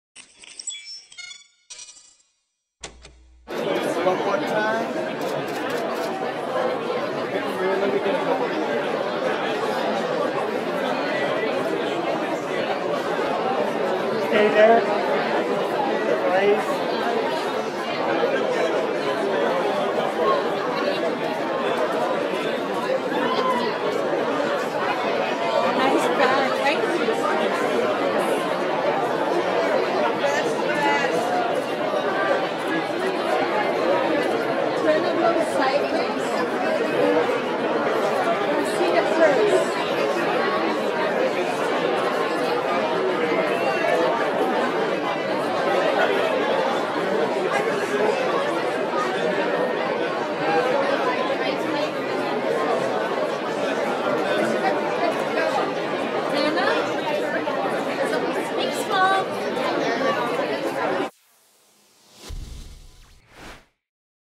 Hannah Marie Hines "Jesus Revolution" Los Angeles Premiere Orange Carpet
Broll footage: Hannah Marie Hines on the orange carpet at Lionsgate's "Jesus Revolution" premiere held at the TCL Chinese 6 Theaters in Los Angeles, California USA on February 15, 2023.

Only In Theaters February 24, 2023 (Wide Release).